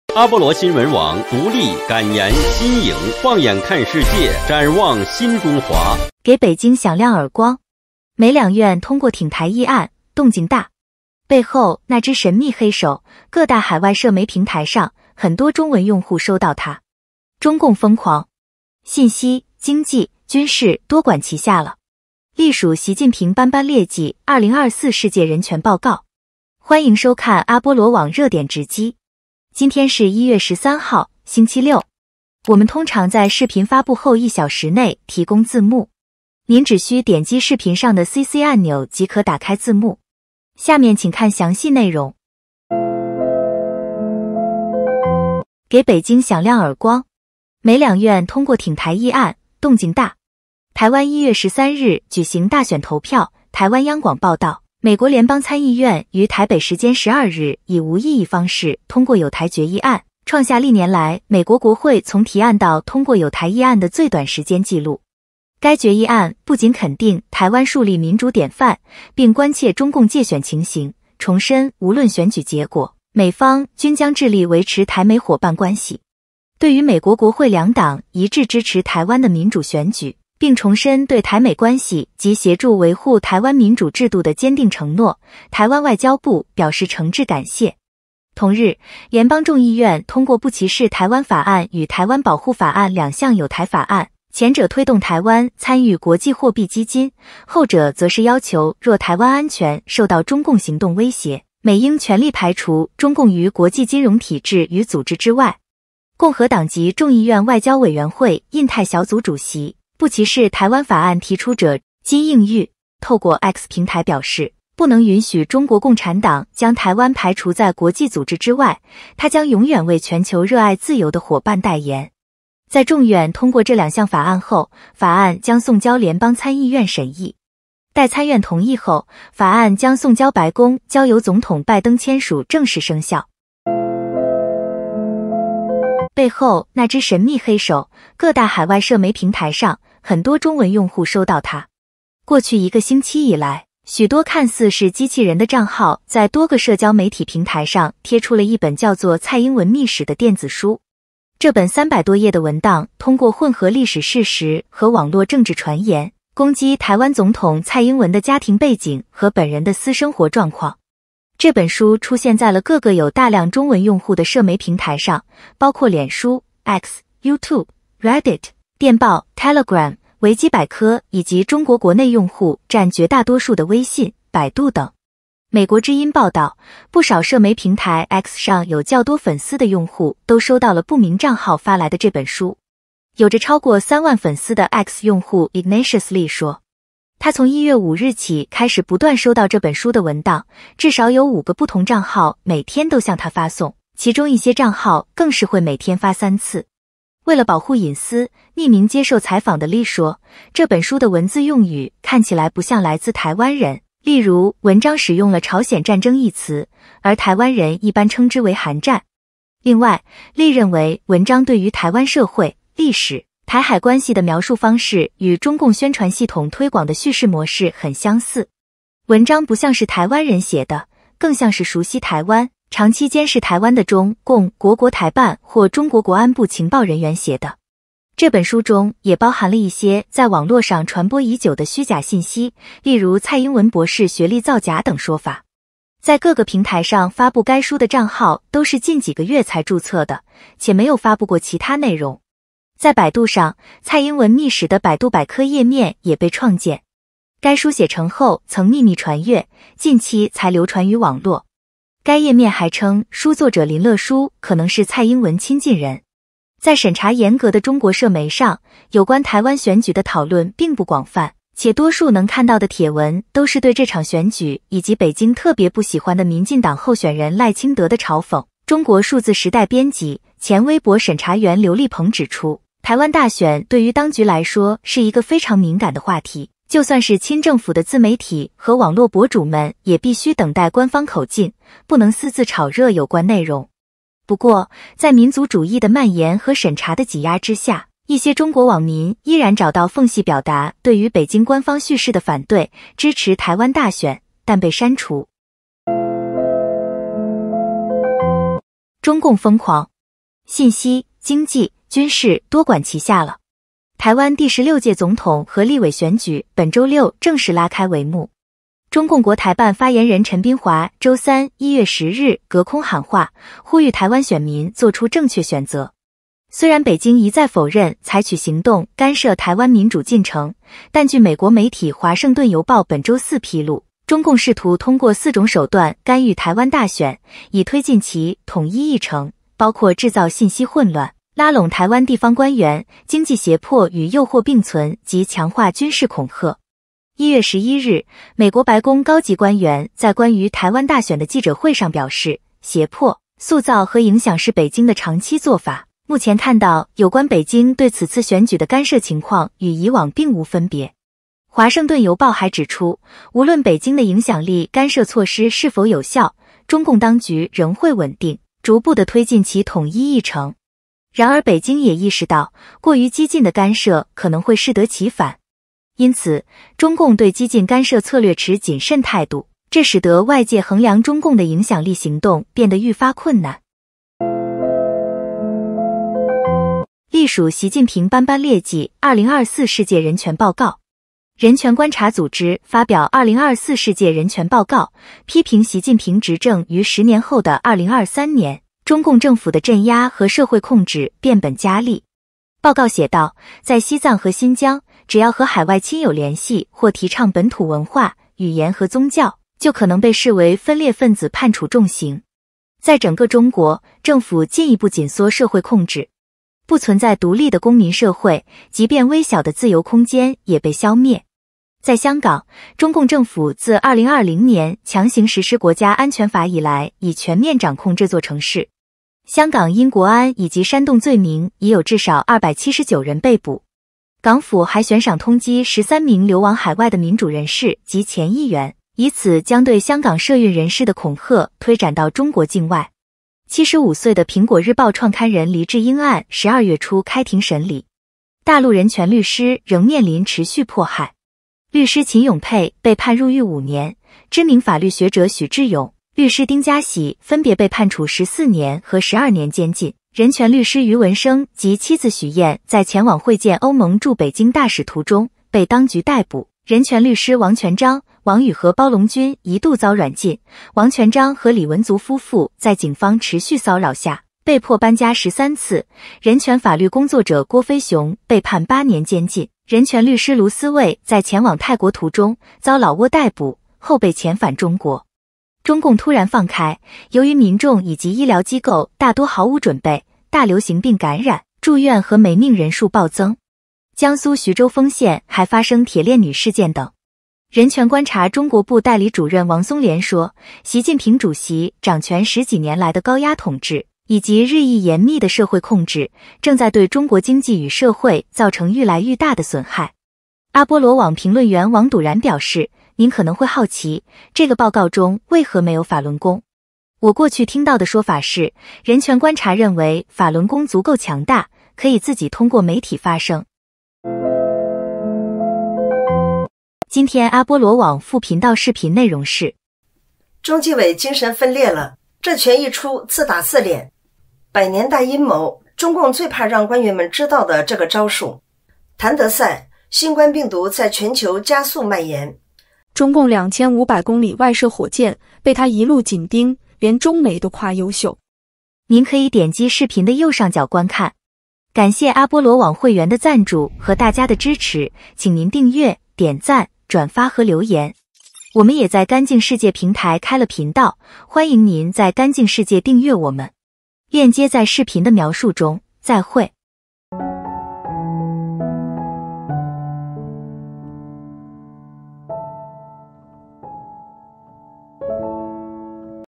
🔥歷數習近平斑斑劣跡 2024世界人權報告；給北京響亮耳光！美兩院通過挺台議案 動靜大；中共瘋狂！信息、經濟、軍事多管齊下了；背後那隻神秘黑手 各大海外社媒平台上 很多中文用戶收到它【阿波羅網WAI】
00:00 摘要
00:45 給北京響亮耳光！美兩院透過挺台議案 動靜大
02:16 背後那隻神秘黑手 各大海外社媒平台上 很多中文用戶收到它
04:58 中共瘋狂！ 資訊、經濟、軍事多管齊下了
06:16 歷數習近平斑斑劣跡 2024世界人權報告
08:36 結束語